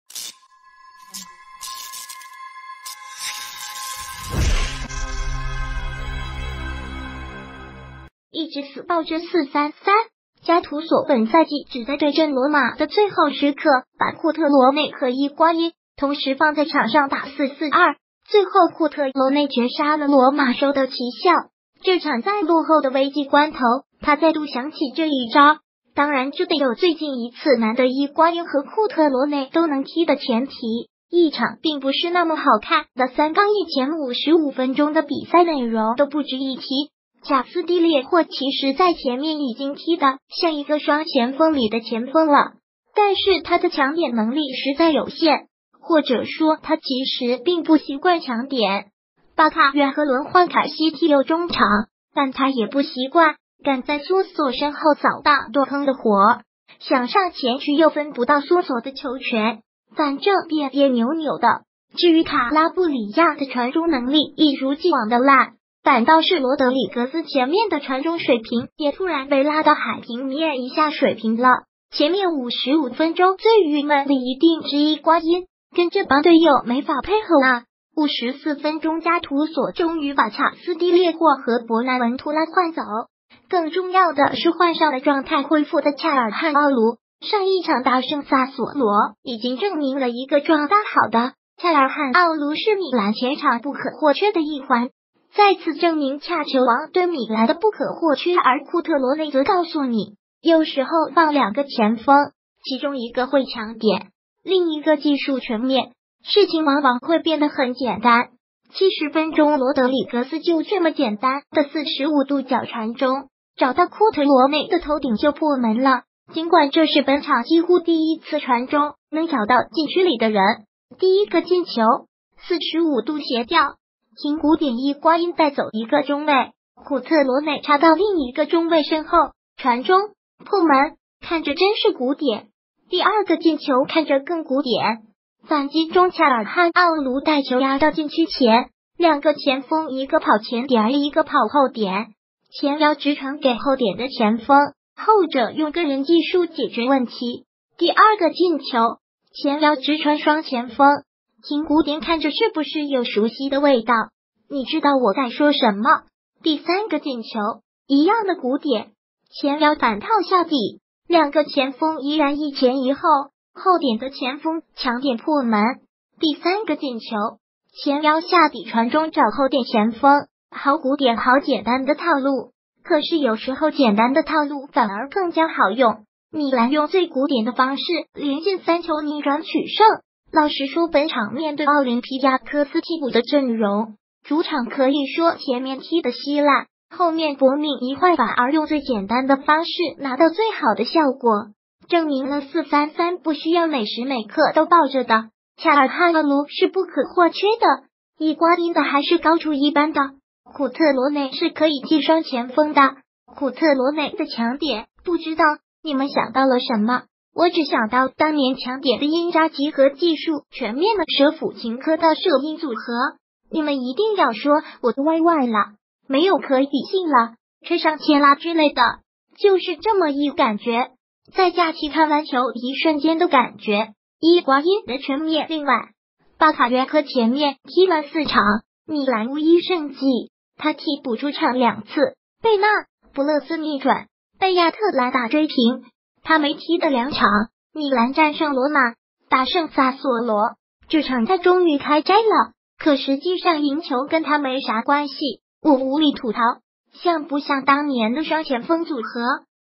米兰用古典的方式赢得比赛 加图索，有没想起当年7号和9号队友？
一直死抱着433，加图索本赛季只在对阵罗马的最后时刻把库特罗内和伊瓜因同时放在场上打442，最后库特罗内绝杀了罗马收到奇效。这场在落后的危机关头他再度想起这一招，当然这得有最近一次难得伊瓜因和库特罗内都能踢的前提。一场并不是那么好看的3-1前55分钟的比赛内容都不值一提。卡斯蒂列霍其实在前面已经踢得像一个双前锋里的前锋了，但是他的抢点能力实在有限，或者说他其实并不习惯抢点。巴卡约科轮换凯西踢右中场，但他也不习惯干在苏索身后扫荡蹲坑的活儿，想上前去又分不到苏索的球权，反正别别扭扭的。至于卡拉布里亚的传中能力一如既往地烂，反倒是罗德里格斯前面的传中水平也突然被拉到海平面一下水平了。前面55分钟最郁闷的一定是伊瓜因，跟这帮队友没法配合啊……54分钟加图索终于把卡斯蒂列霍和博纳文图拉换走。更重要的是换上了状态恢复的恰尔汗奥卢，上一场大胜萨索罗已经证明了一个状态好的恰尔汗奥卢是米兰前场不可或缺的一环。再次证明恰球王对米兰的不可或缺而库特罗内则告诉你，有时候放两个前锋，其中一个会抢点，另一个技术全面。事情往往会变得很简单。70分钟罗德里格斯就这么简单的45度角传中，找到库特罗内的头顶就破门了——尽